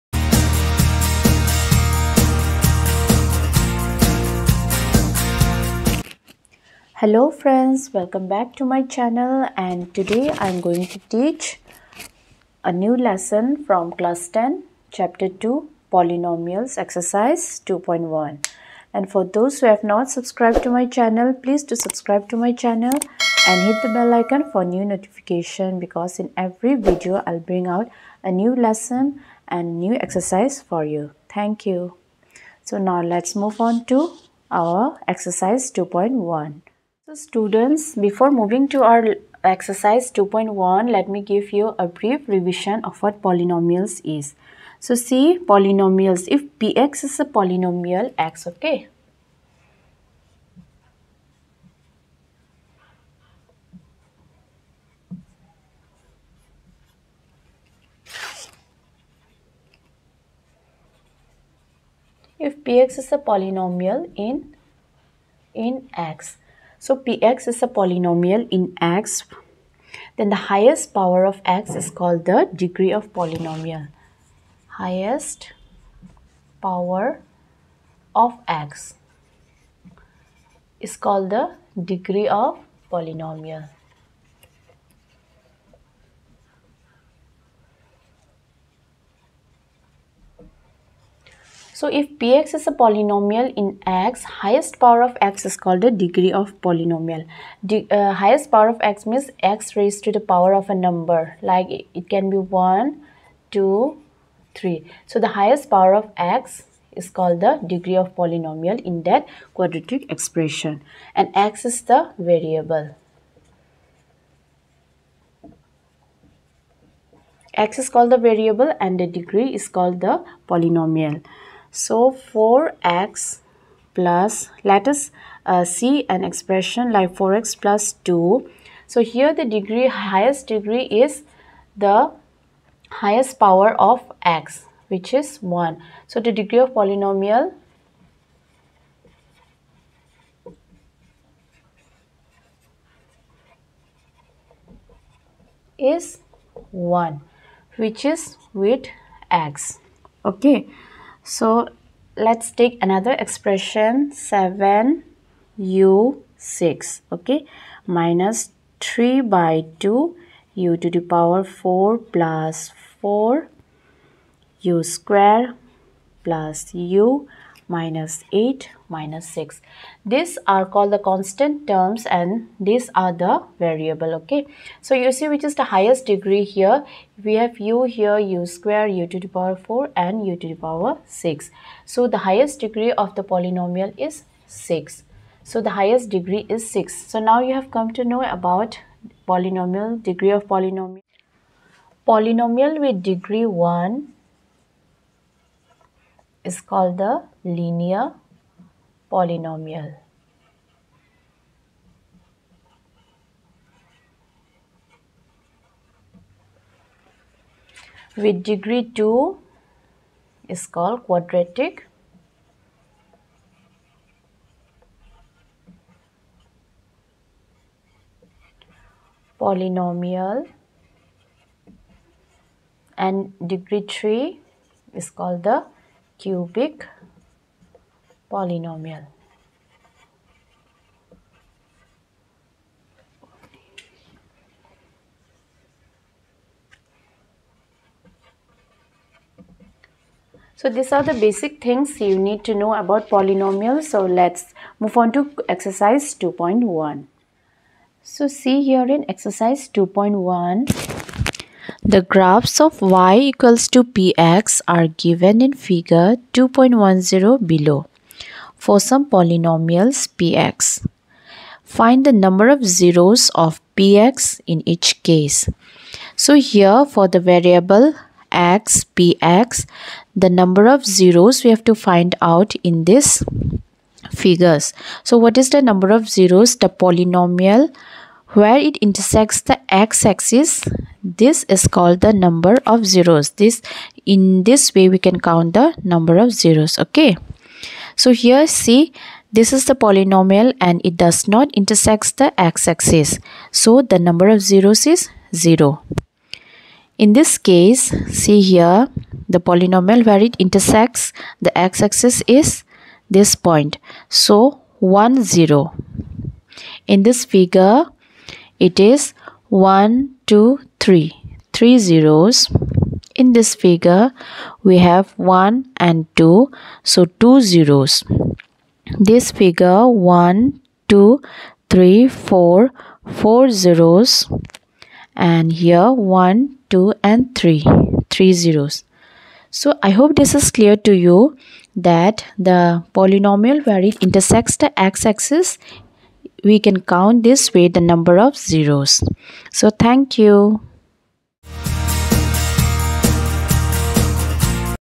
Class 10 Maths / Chapter 2 / Polynomials /Exercise 2.1
In the above video I have solved Question no 1 from Chapter 2 (Polynomials) of Class 10.

About Polynomials

Degree of Polynomials

Linear Polynomial

Quadratic Polynomial

Cubic Polynomial

Zeroes of Polynomial

Drop your valuable Comments below in the Comments Section.